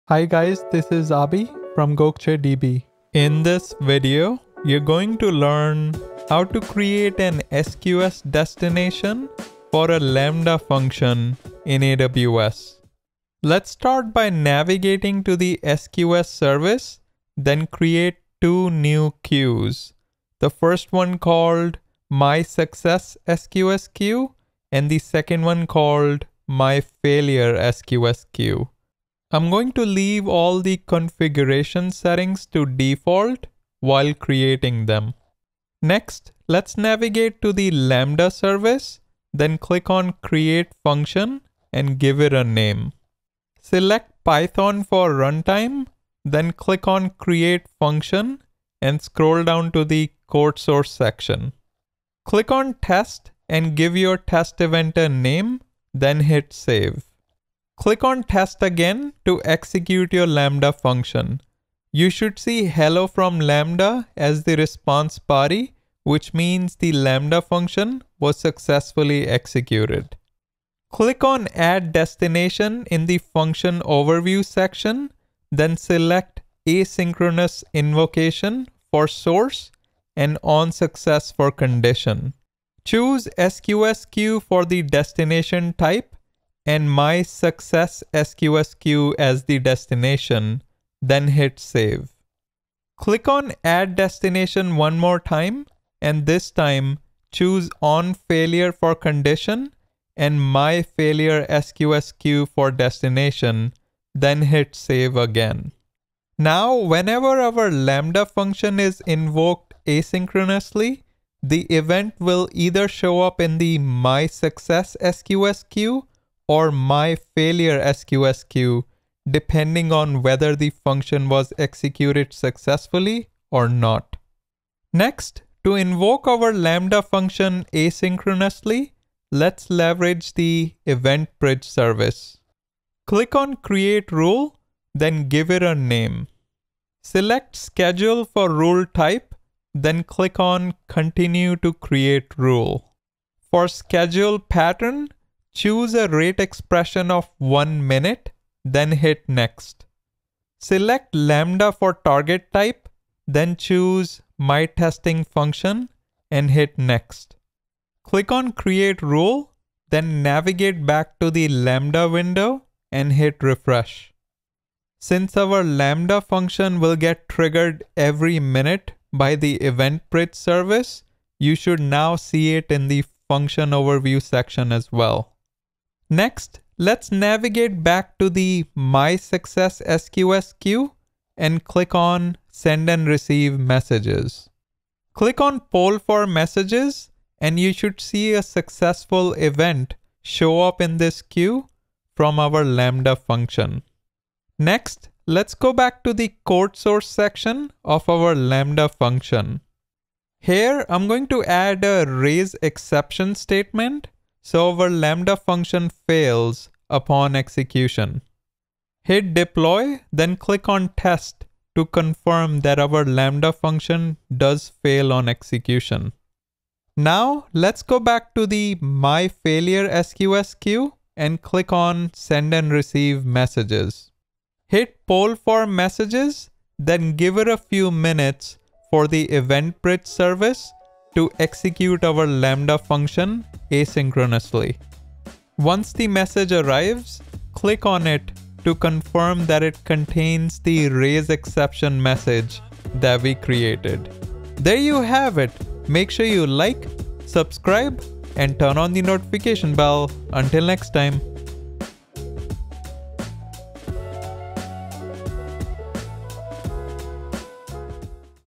How To: SQS Destination For Lambda (4 Min) | AWS | Asynchronous Invocation Using Event Bridge
In this tutorial, you'll learn how to invoke Lambda function asynchronously using Event Bridge and set SQS as the destination for both success and failure conditions.
Video Transcript:
Hi guys, this is Abhi from Gokcedb. In this video, you're going to learn how to create an SQS destination for a Lambda function in AWS. Let's start by navigating to the SQS service and then create two new cues. The first one is called My Success SQSQ and the second one is called my failure SQSQ.

I'm going to leave all the configuration settings to default while creating them. Let's navigate to the Lambda service then click on create function and give it a name. Select Python for runtime then click on create function and scroll down to the code Source section.

Click on test and give your test event a name then hit save. Click on the test again to execute your Lambda function. You should see hello from Lambda as the response body which means the Lamba function was successfully executed.

Click on ADD destination in the function overview section then select asynchronous invocation for source and on success for condition. Choose SQSQ for the Destination type and my success SQSQ as the destination then hit save. Click on ADD destination one more time and this time choose failure for condition and my failure SQSQ for Destination then hit save again.

Now whenever our Lambda function is invoked asynchronously, the event will either show up in the My Success SQSQ or my failure SQSQ depending on whether the function was executed successfully or not. Next, to invoke our Lambda function asynchronously, let's leverage the event bridge service. Click on create rule then give it a name.

Select schedule for role type then clicks on continue to create a rule for schedule pattern. Choose a rate expression of one minute then hit next. Select Lambda for the Target type then choose my testing function and hit next.

Click on create rule then navigate back to the Lambda window and hit refresh. Since our Lambda function will get triggered every minute by the event bridge service, you should now see it in the function overview section as well. Next, let's navigate back to the My Success SQS queue and click on send and receive messages.

Click on Poll for messages and you should see a successful event show up in this queue from our Lambda function. Next, let's go back to the code Source section of our Lambda function. Here I'm going to add a raise exception statement server Lambda function fails upon execution.

Hit deploy then clickon test to confirm that our Lambda function does fail on execution. Now, let's go back to my failure SQSQ and click on send and receive messages. Hit Poll for messages then gives it a few minutes for the event bridge service to execute our Lambda function asynchronously.

Once the message arrives, click on it to confirm that it contains the raise exception message that we created. There you have it.